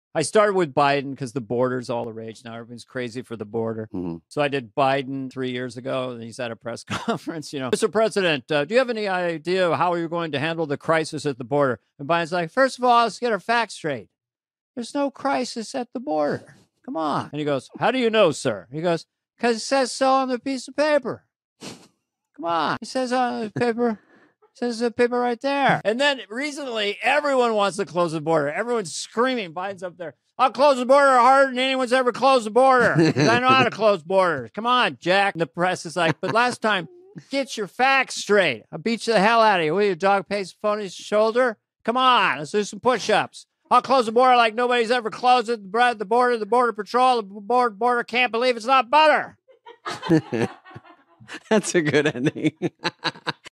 Dana Carvey Does The Best Biden Impression
Dana Carvey, known for his uncanny political impressions, has garnered attention for his portrayal of President Joe Biden. Carvey's comedic genius shines as he captures Biden's mannerisms and speech patterns with precision, adding humor to the political landscape. Through his impressions, Carvey injects levity into discussions surrounding Biden's presidency, offering audiences a comedic lens through which to view current events.

👉 Why BlabberBuzz?

Stay Informed: From politics to tech, we cover what matters.